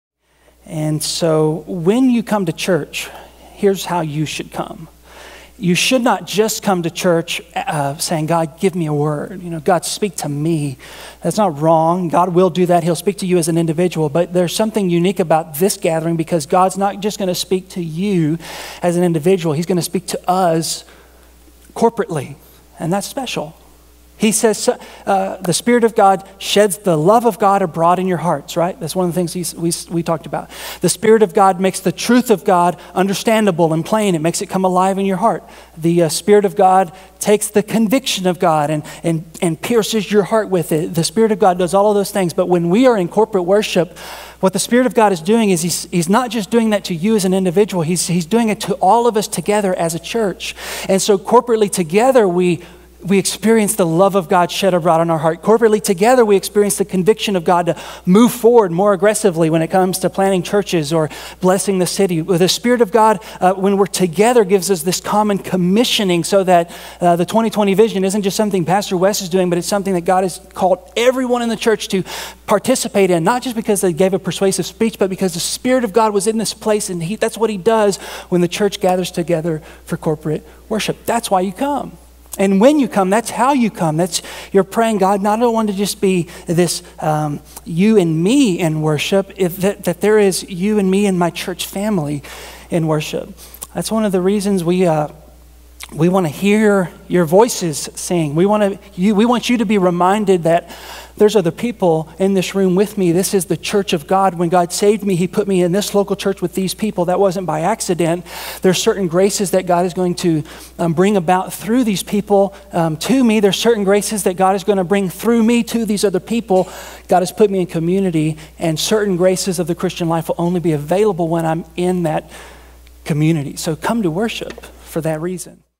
Why Should We Worship Together?
This is a clip from the message "Spirit Empowered Worship." In this message, Lead Pastor Wes Terry talks about what it means to have "Spirit Empowered Worship" out of Acts 2:42-47. This message is part of the series "Empowered By The Spirit." The message was given on August 21, 2016.